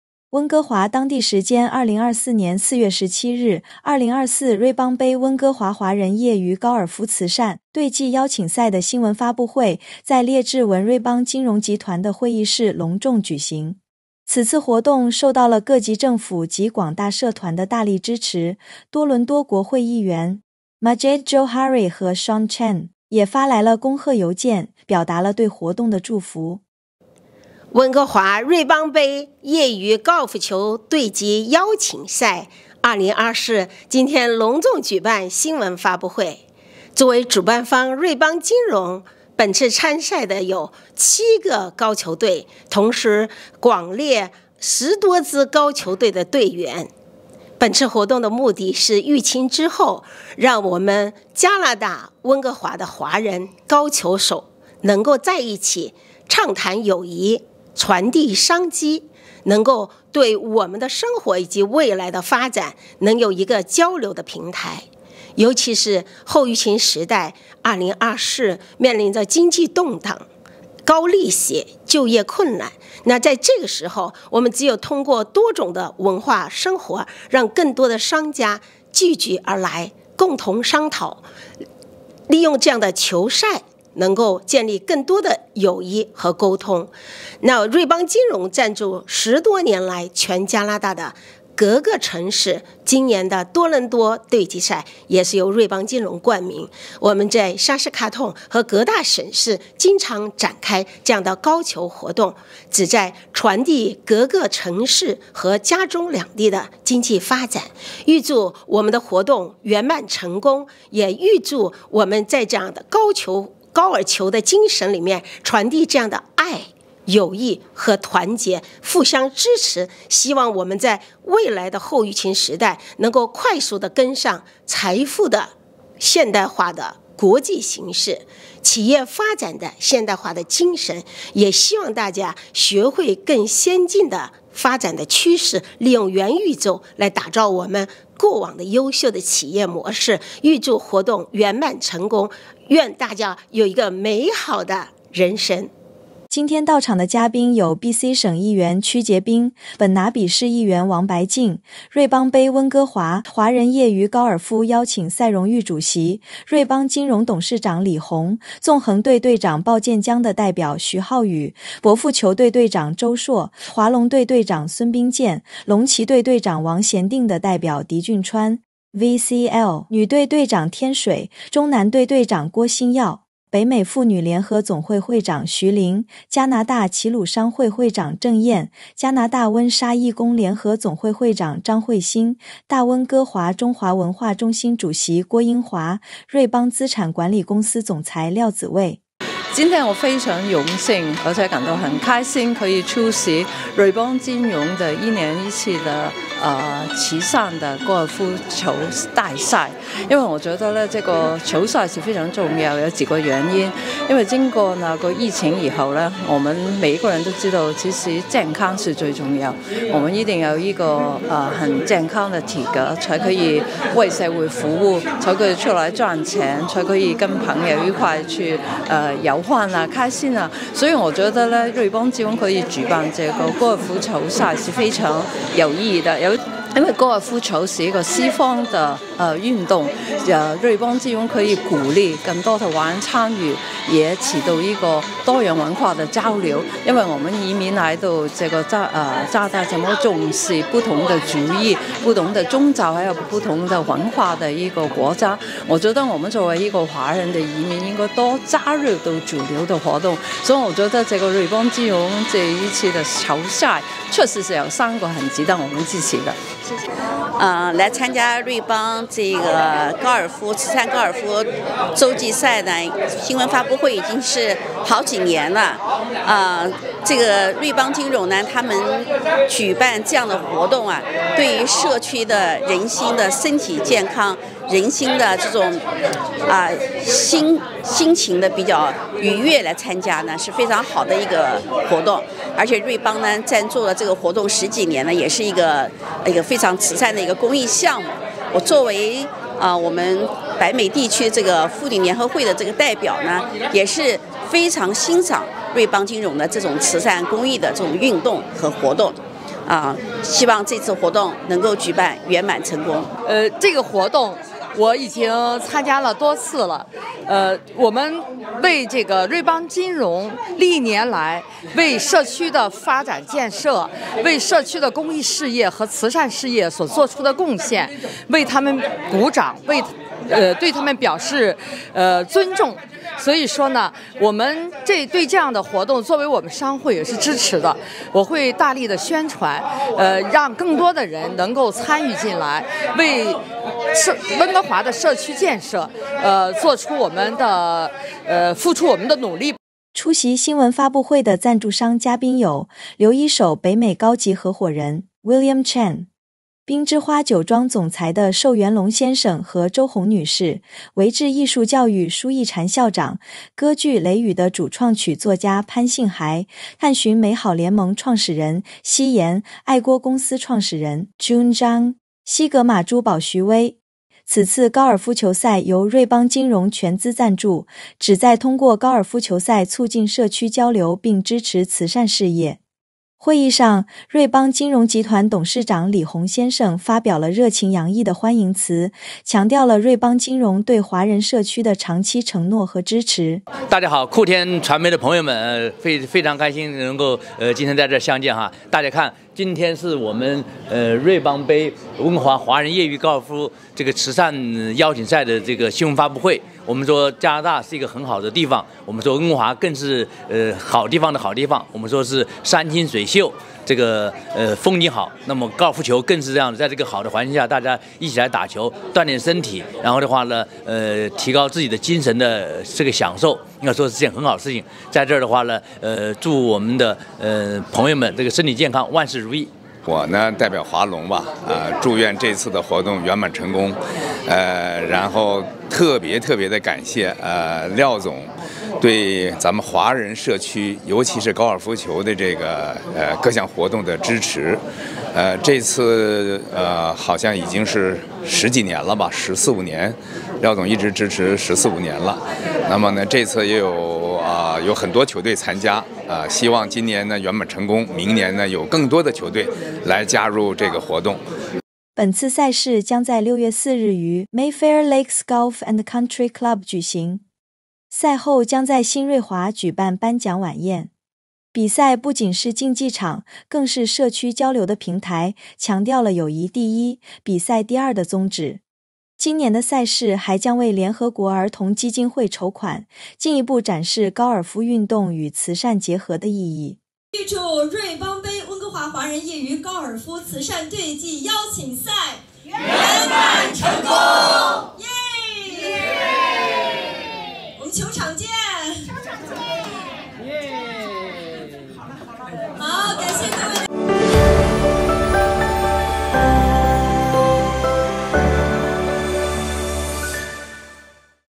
2024瑞邦杯温哥华华人业余高尔夫慈善队际邀请赛新闻发布会圆满举行
2024年4月17日，列治文——2024瑞邦杯温哥华华人业余高尔夫慈善队际邀请赛的新闻发布会于瑞邦金融集团的会议室隆重举行。这一备受期待的赛事得到了各级政府和广大社团的大力支持，多伦多国会议员Majid Jowhari和Shaun Chen也通过恭贺邮件表达了对活动的祝福。

出席发布会的嘉宾包括BC省省议员屈洁冰、本拿比市议员James Wang、瑞邦金融董事长李洪，以及多个赞助商和社区领袖。瑞邦金融集团董事长李洪在致欢迎词时，再次强调了对华人社区的长期承诺和支持。

本届赛事将于6月4日在Mayfair Lakes Golf and Country Club举行，并于当天晚上在新瑞华举办颁奖晚宴。赛事以“友谊第一，比赛第二”为宗旨，旨在通过体育活动增强社区联系，并通过慈善捐款支持联合国儿童基金会，进一步展示高尔夫运动的社会价值。

本次赛事的成功举行预示着温哥华华人社区的体育与慈善活动将继续发展壮大，为华人社区的幸福生活增添色彩。